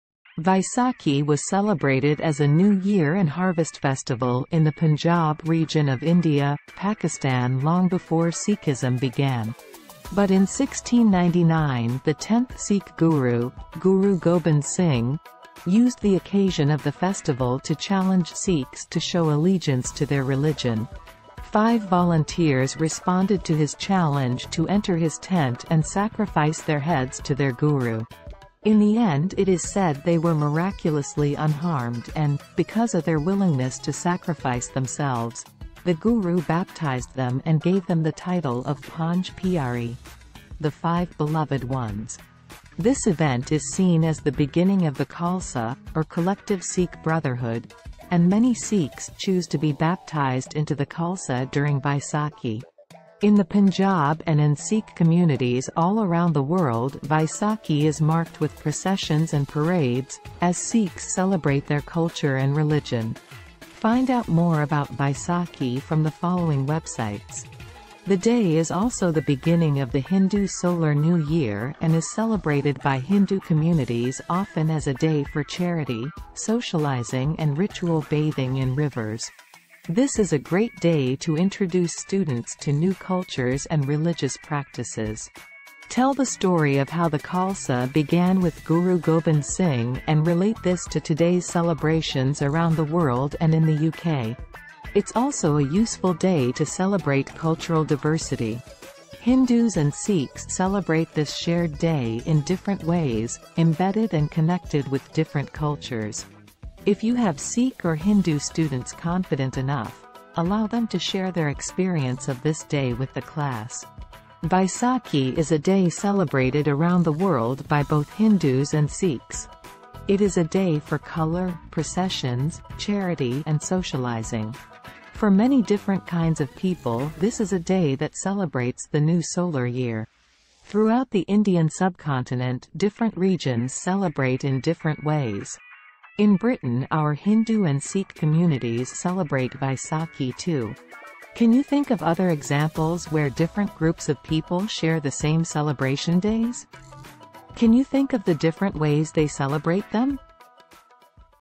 Vaisakhi Taking place on 13th or 14th April every year
Vaisakhi was celebrated as a new year and harvest festival in the Punjab region of India/Pakistan long before Sikhism began. But in 1699 the tenth Sikh Guru, Guru Gobind Singh, used the occasion of the festival to challenge Sikhs to show allegiance to their religion. Five volunteers responded to his challenge to enter his tent and sacrifice their heads to their Guru. In the end it is said they were miraculously unharmed and, because of their willingness to sacrifice themselves, the Guru baptised them and gave them the title of Panj Piare ("the five beloved ones").